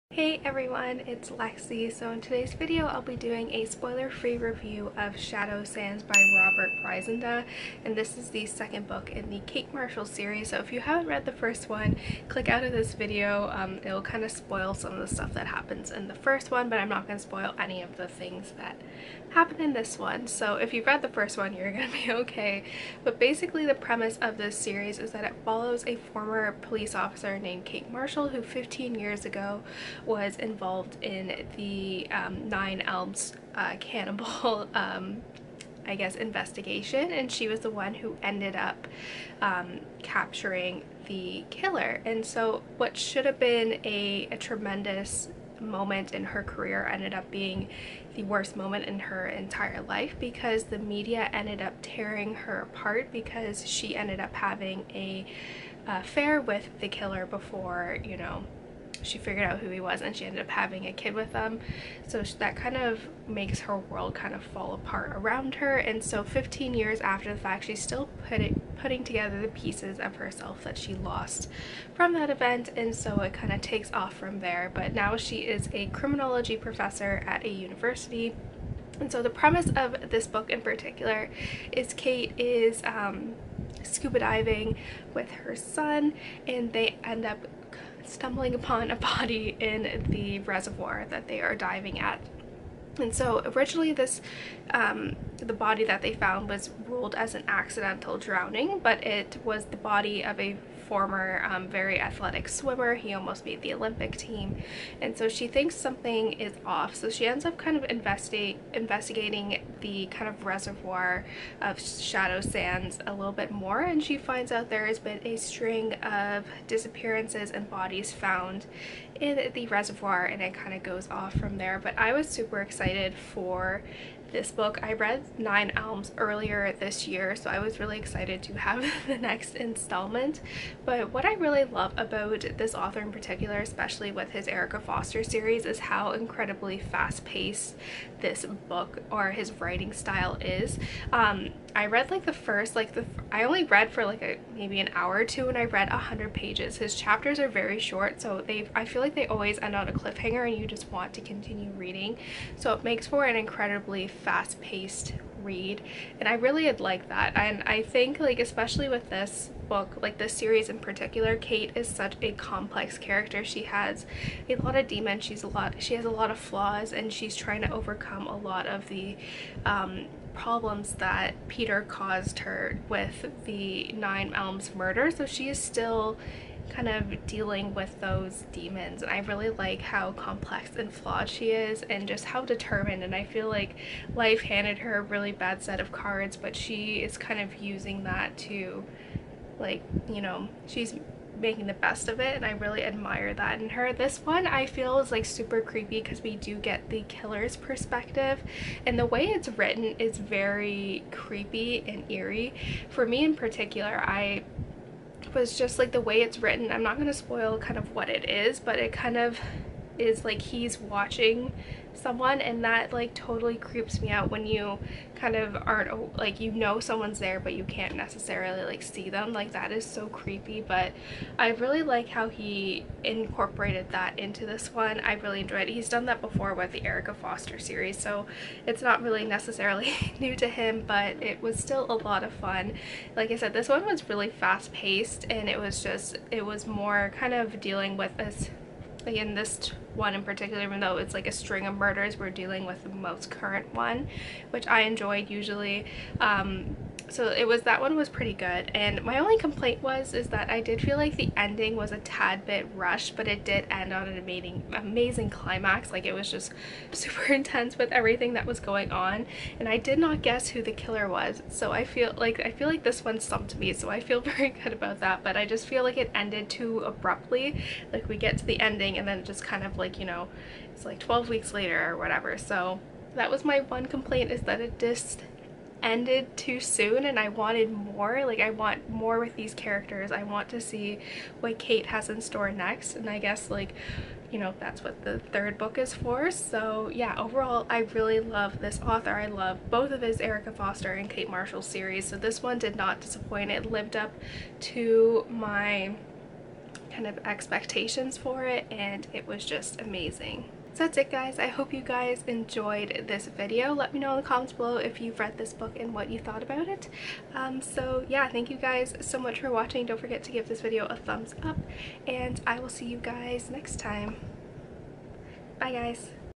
Shadow Sands - Robert Bryndza | Review (Spoiler-Free!)
Here are all my thoughts and opinions on "Shadow Sands" by Robert Bryndza. Enjoy!

Letterboxd: @lexiedm14